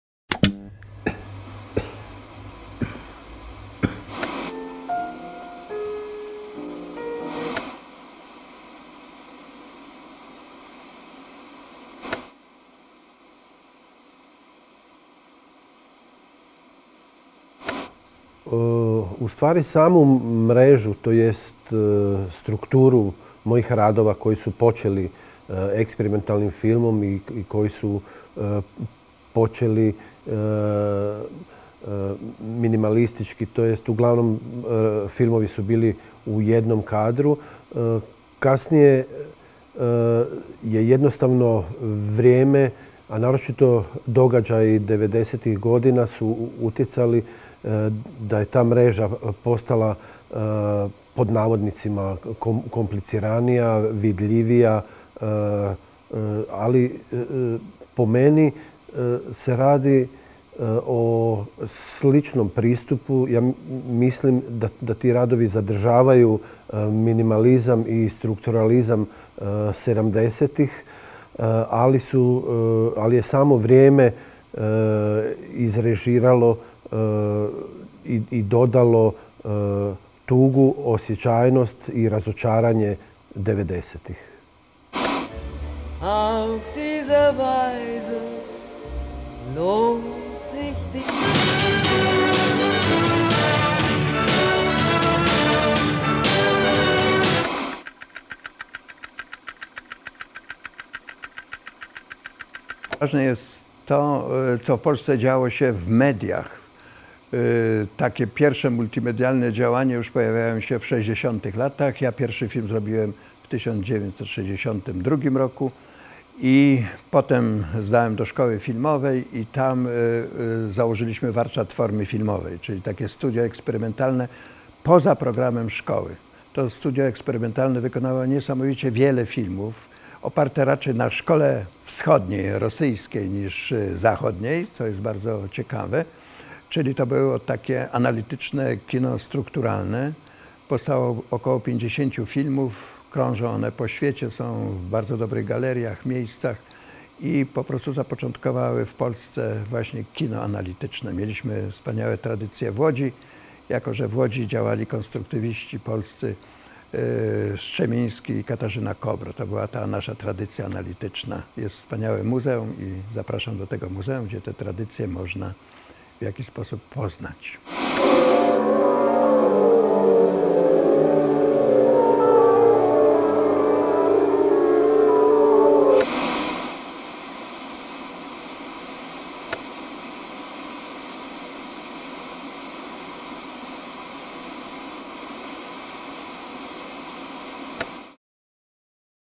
Ivan Faktor & Józef Robakowski
Mit den beiden Einzelausstellungen »Ivan Faktor. Erstes Programm« und »Józef Robakowski. The Handshake« zeigt das ZKM | Karlsruhe bis zum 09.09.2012 die Medienpioniere Ivan Faktor und Józef Robakowski. In Faktors Foto- und Videoarbeiten wie in Robakowskis Experimentalfilmen kommt der avantgardistische Anspruch beider Künstler zum Ausdruck. Robakowski, Initiator der »Exchange Gallery«, einem politisch-engagierten Künstlerforum und Faktor, Vorreiter auf dem Gebiet der Montagetechnik, gelten als Schlüsselfiguren der osteuropäischen Techno-Avantgarde der Künste.